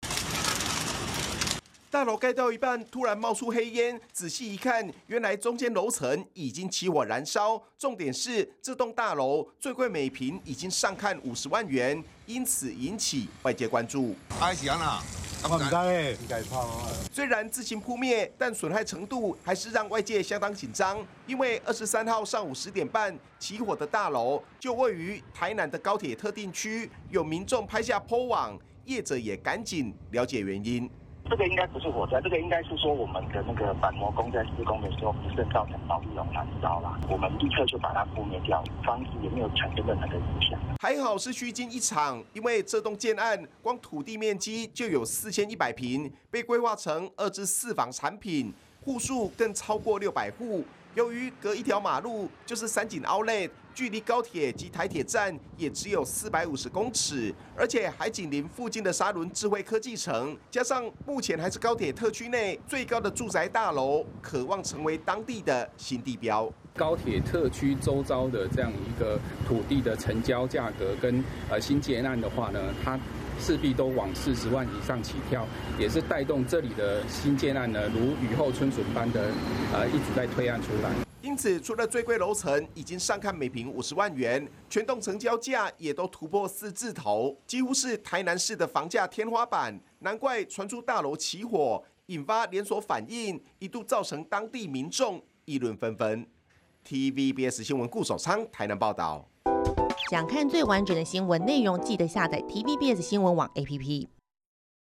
台南高鐵特區新地標起火? 業者:誤會啦 板模工誤燒保麗龍｜TVBS新聞
台南高鐵特區23號上午十點半左右，一棟興建中的大樓，突然竄出火苗，消防局至少出動五個分隊前往搶救，還好火勢很快就被撲滅，但由於大樓距離知名的三井OUTLET只有一路之隔，加上房價最高已經上看每坪50萬元，因此一度引起外界關注。